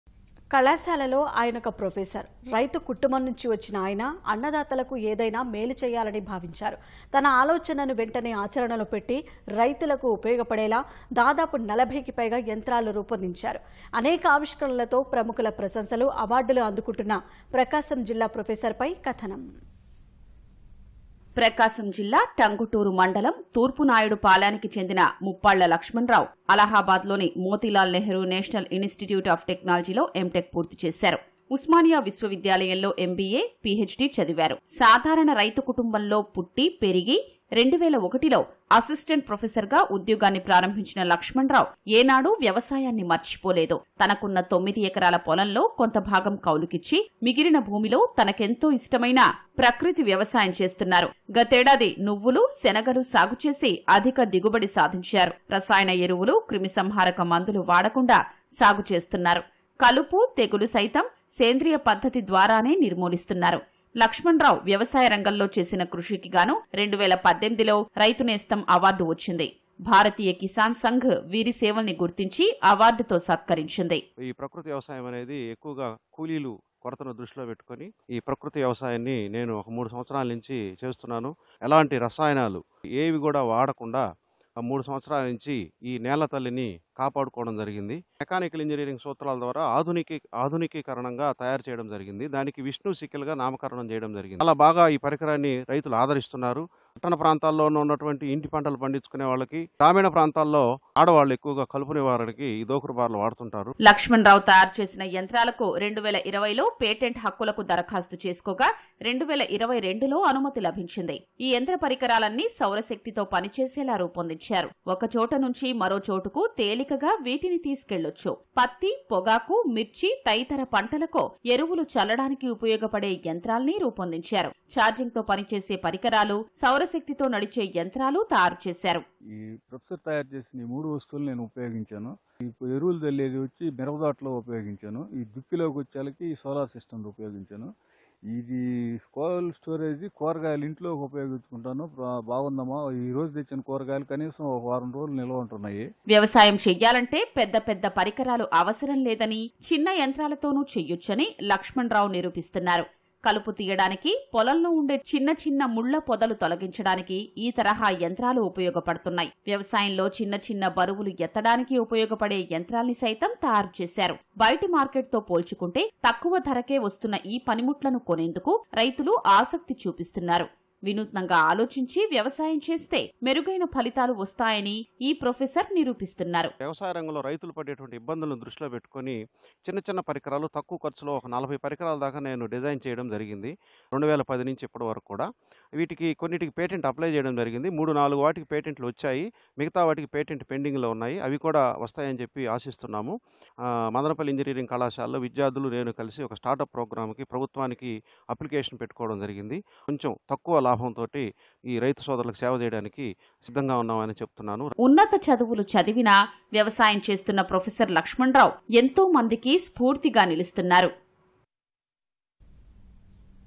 A Special Story on Professor Muppalla Lakshman Rao | Organic Farming | Thurpu Naidu Palem
కళాశాలలో ఆయనొక ప్రొఫెసర్. రైతు కుటుంబం నుంచి వచ్చిన ఆయన. అన్నదాతలకు ఏదైనా మేలు చేయాలని భావించారు. తన ఆలోచనను వెంటనే ఆచరణలో పెట్టి... రైతులకు ఉపయోగపడేలా దాదాపు 40కి పైగా యంత్రాలు రూపొందించారు. అనేక ఆవిష్కరణలతో ప్రముఖుల ప్రశంసలు, అవార్డులు అందుకుంటున్న ప్రకాశం జిల్లా ప్రొఫెసర్ పై కథనం.